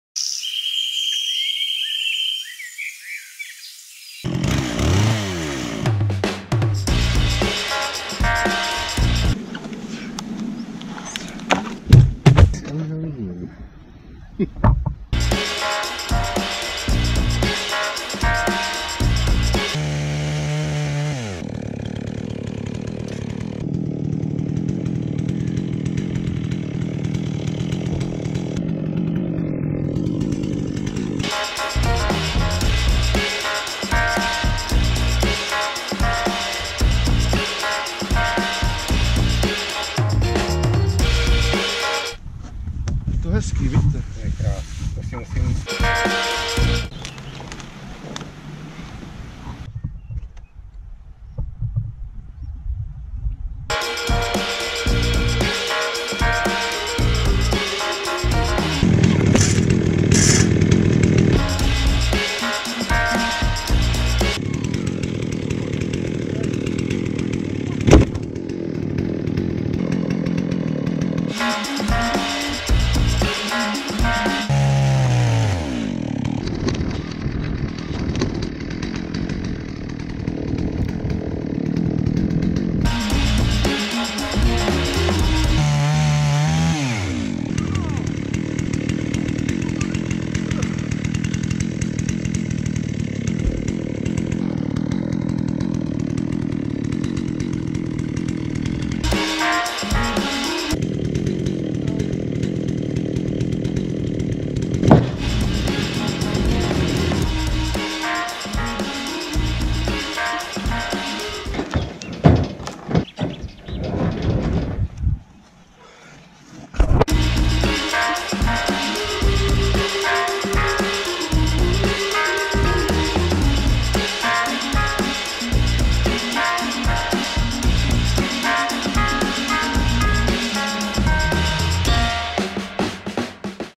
Woodworking: Milling lumber Slabs for Epoxy! Beautiful Walnut
Milling amazing english Walnut Logs into slabs with my basic chainsaw mill.
I am using the Holzfforma G660 here, a Stihl 660 clone, holds good for now.
Enjoy the short video and leave a like or comment bellow!